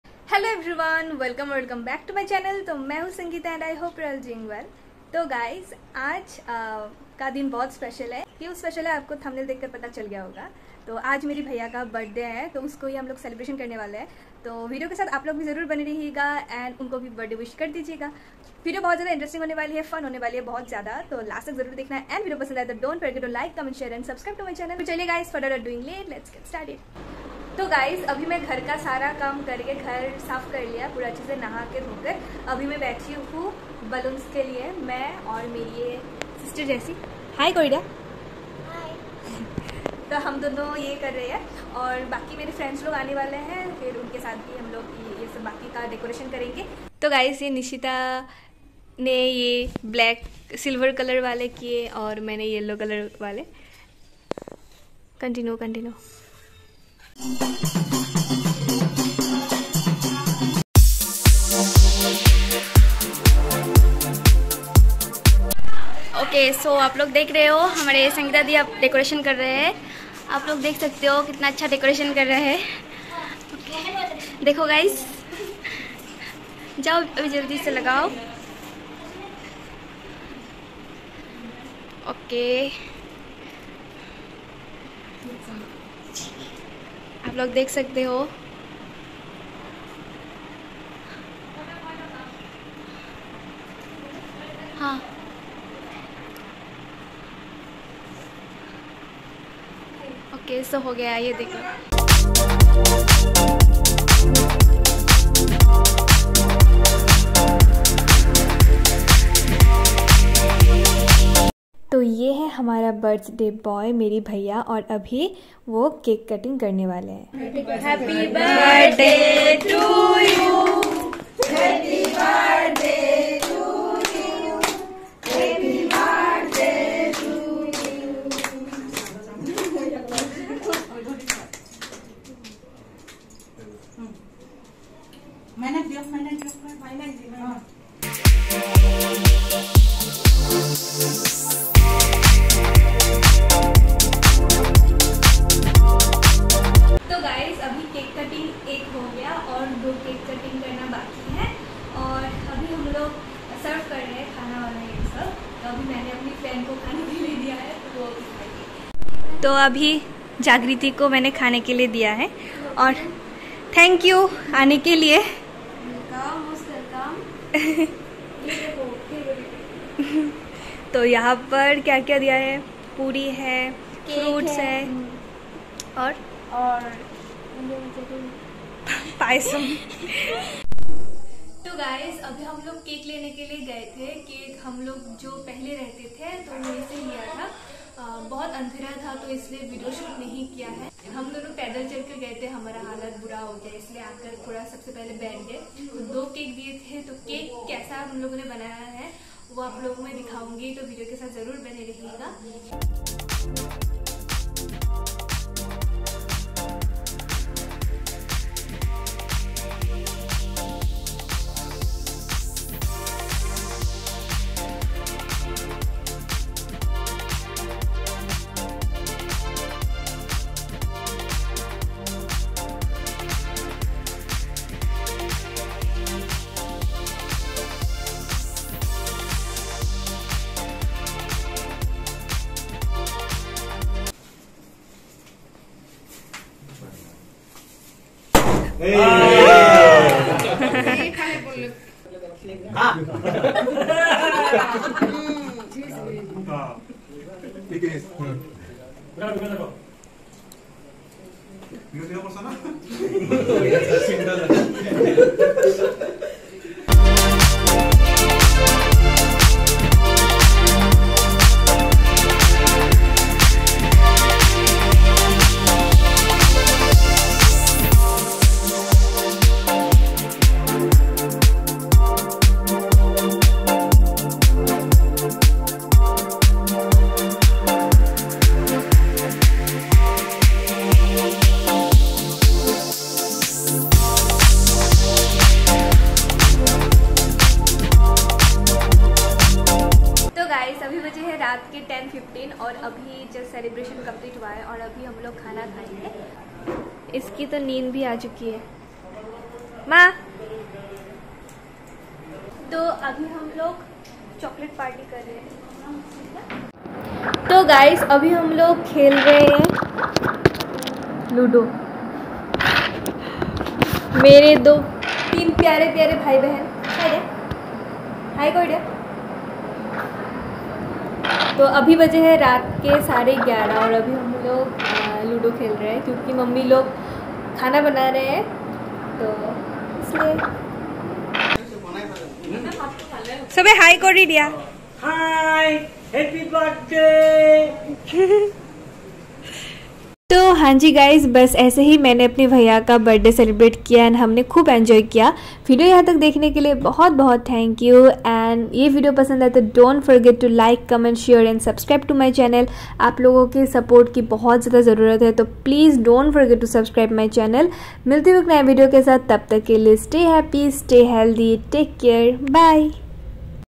Birthday Celebration Of My Brother 💐🥳
Here I have uploaded new vlog of my brother's Birthday 🎂🎆